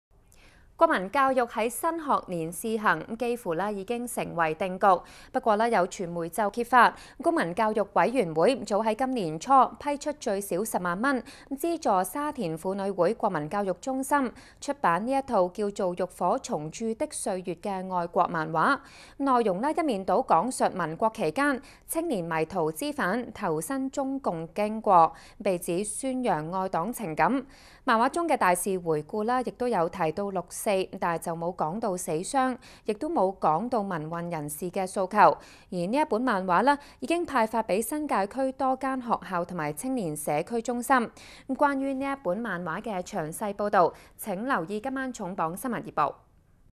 國教漫畫早滲學校圖書館
國民教育在新學年試行，幾乎成為定局。有傳媒揭發，公民教育委員會早在今年初批出最少十萬元，資助沙田婦女會國民教育中心。